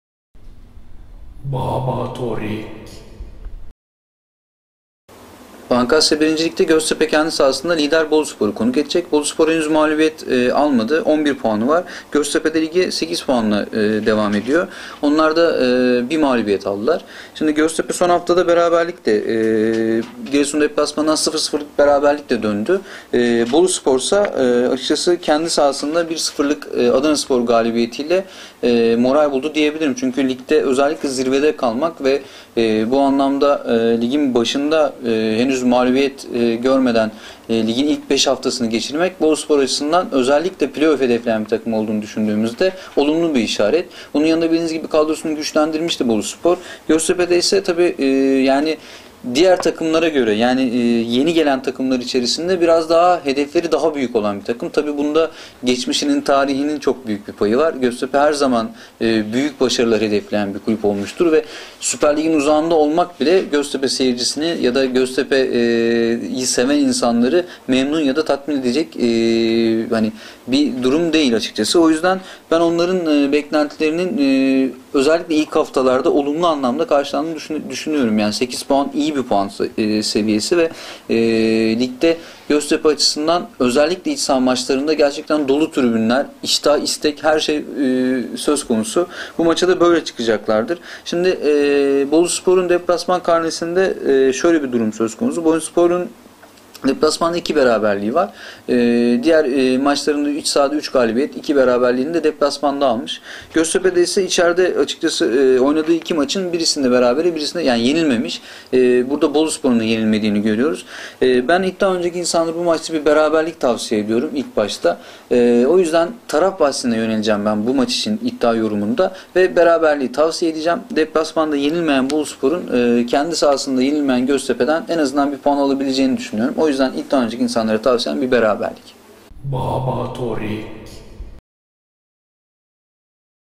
Babatorik Goztepe Boluspor 16 Ekim 2011
ilker anlatiyor 16 Ekim 2011 Goztepe Boluspor
Bolu nagmaglup unvanini surdurebilecek mi?